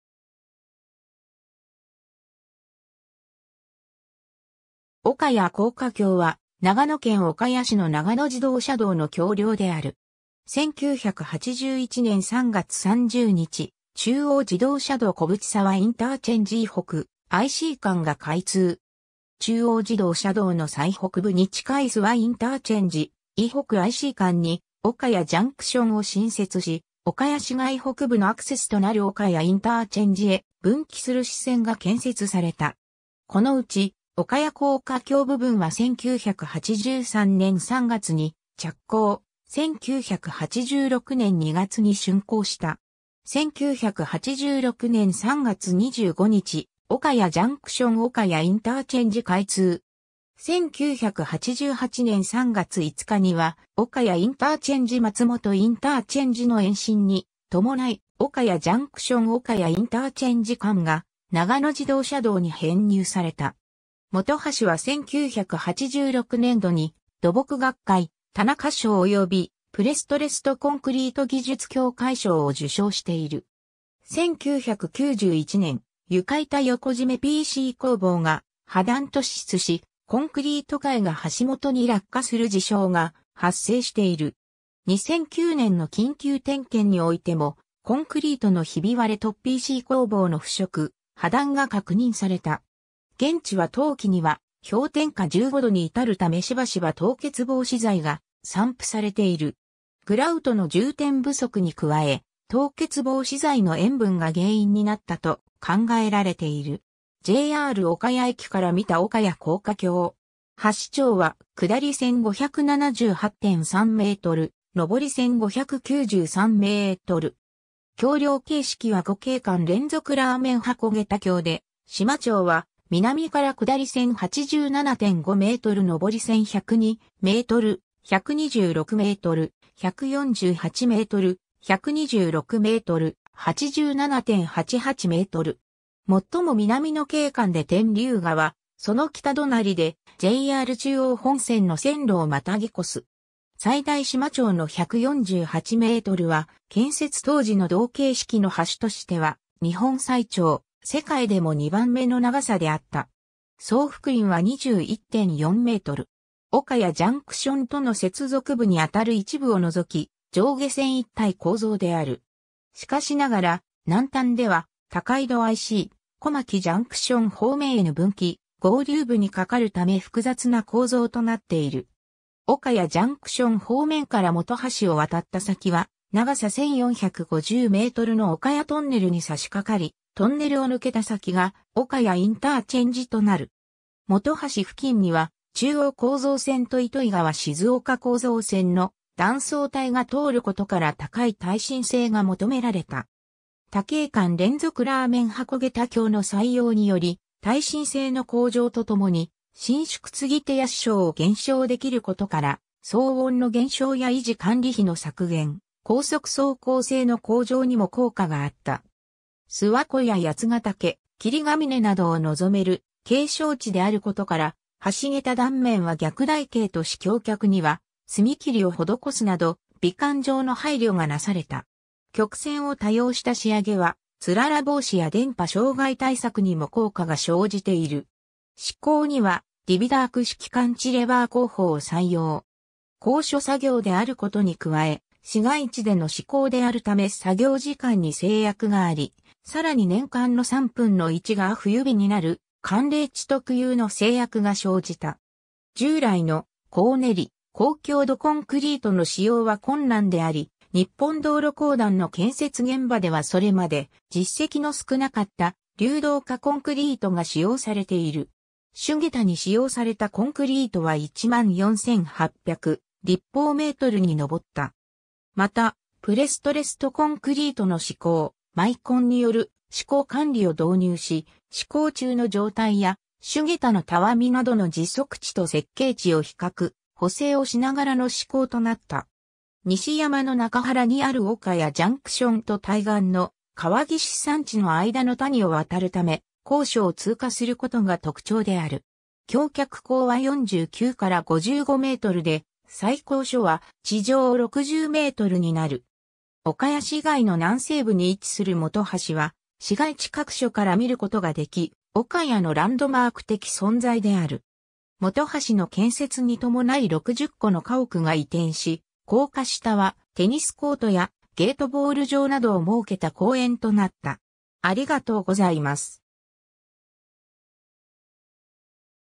岡谷高架橋
岡谷高架橋（おかやこうかきょう）は、長野県岡谷市の長野自動車道の橋梁である。
 1981年（昭和56年）3月30日、中央自動車道小淵沢IC - 伊北IC間が開通。
中央自動車道の最北部に近い諏訪IC - 伊北IC間に岡谷JCTを新設し、岡谷市街北部のアクセスとなる岡谷ICへ分岐する支線が建設された。
このうち、岡谷高架橋部分は1983年（昭和58年）3月に着工、1986年（昭和61年）2月に竣工した。
 1986年（昭和61年）3月25日、岡谷JCT - 岡谷IC開通。
1988年（昭和63年）3月5日には岡谷IC - 松本ICの延伸に伴い、岡谷JCT - 岡谷IC間が長野自動車道に編入された。
 本橋は1986年度に土木学会田中賞およびプレストレストコンクリート技術協会賞を受賞している。
 1991年、床板横締めPC鋼棒（ディビダーク鋼棒）が破断突出し、コンクリート塊が橋下に落下する事象が発生している。
2009年の緊急点検においても、コンクリートのひび割れとPC鋼棒の腐食・破断が確認された。
現地は冬季には氷点下15度に至るためしばしば凍結防止剤が散布されている。
グラウトの充填不足に加え、凍結防止剤の塩分が原因になったと考えられている。
 JR岡谷駅から見た岡谷高架橋。
 橋長は下り線578.3 m、上り線593 m。
橋梁形式は5径間連続ラーメン箱桁橋で、支間長は南から下り線87.5 m・上り線102 m、126 m、148 m、126 m、87.88 m。
最も南の径間で天竜川、その北隣でJR中央本線の線路をまたぎ越す。
最大支間長の148 mは、建設当時の同形式の橋としては日本最長、世界でも2番目の長さであった。
総幅員は21.4m（車道幅員9m×2）。
岡谷JCTとの接続部にあたる一部を除き、上下線一体構造である。
しかしながら、南端では高井戸IC・小牧JCT方面への分岐・合流部にかかるため複雑な構造となっている。
岡谷JCT方面から本橋を渡った先は長さ1,450 m（上り線は1,390 m）の岡谷トンネルに差し掛かり、トンネルを抜けた先が岡谷ICとなる。
 本橋付近には中央構造線と糸魚川静岡構造線の断層帯が通ることから高い耐震性が求められた。
多径間連続ラーメン箱桁橋の採用により、耐震性の向上とともに、伸縮継手や支承を減少できることから騒音の減少や維持管理費の削減、高速走行性の向上にも効果があった。
諏訪湖や八ヶ岳、霧ヶ峰などを望める景勝地であることから、橋桁断面は逆台形とし橋脚には隅切りを施すなど美観上の配慮がなされた。
曲線を多用した仕上げは、つらら防止や電波障害対策にも効果が生じている。
 施工にはディビダーク式カンチレバー工法を採用。
高所作業であることに加え、市街地での施工であるため作業時間に制約があり、さらに年間の3分の1が冬日になる寒冷地特有の制約が生じた。
従来の硬練り高強度コンクリートの使用は困難であり、日本道路公団の建設現場ではそれまで実績の少なかった流動化コンクリートが使用されている。
主桁に使用されたコンクリートは14800立方メートルにのぼった。
また、プレストレストコンクリートの施工、マイコンによる施工管理を導入し、施工中の状態や主桁のたわみなどの実測値と設計値を比較・補正をしながらの施工となった。
 西山の中腹にある岡谷JCTと対岸の川岸山地の間の谷を渡るため、高所を通過することが特徴である。
橋脚高は49～55mで、最高所は地上60mになる。
岡谷市街の南西部に位置する本橋は市街地各所から見ることができ、岡谷のランドマーク的存在である。
本橋の建設に伴い60戸の家屋が移転し、高架下はテニスコートやゲートボール場などを設けた公園となった。